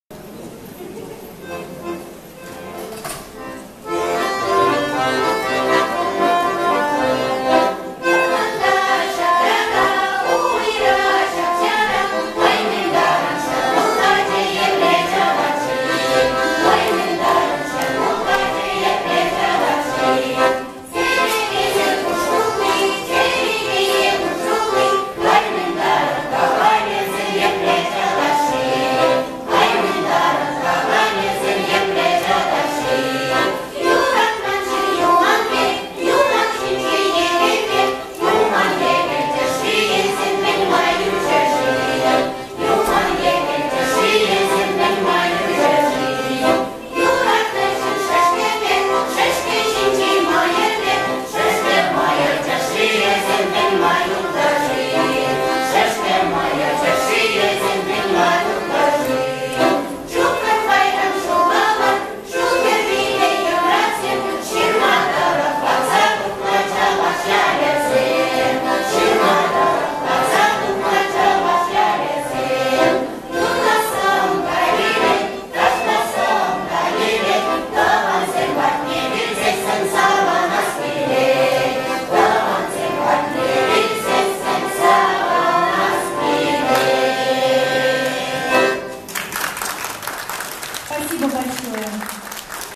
2021г. Фестиваль- конкурс творчества пенсионеров. ч.53. Мариинский Посад
2021г. 6 апреля. Дом культуры